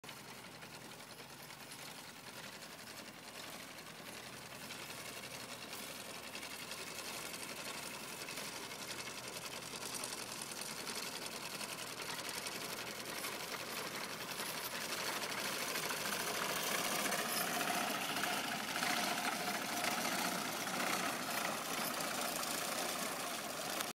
Mowing Hay with Draft Horses
Using Draft power to mow gay. Soleil and Simone work in unison to get the job done.